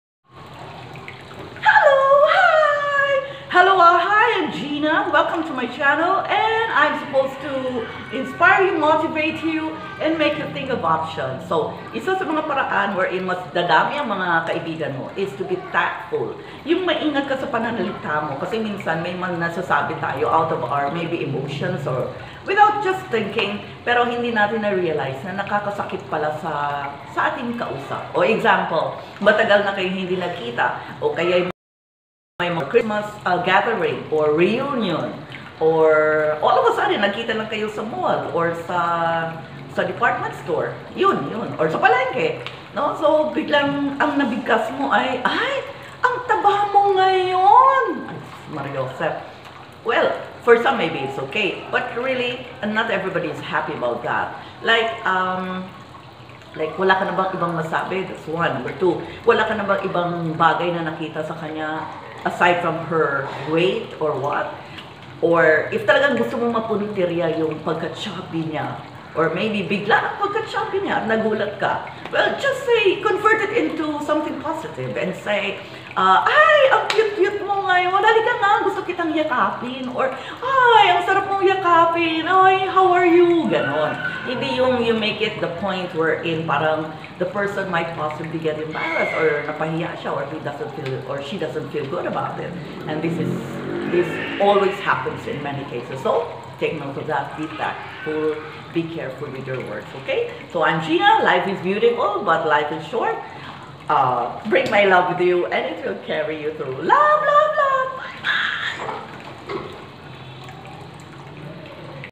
How To Be Tactful When Meeting a Friend Who Gained Weight (Paano Batiin ang Kaibigang Tumaba)
A short video on how to greet friends who gained weight.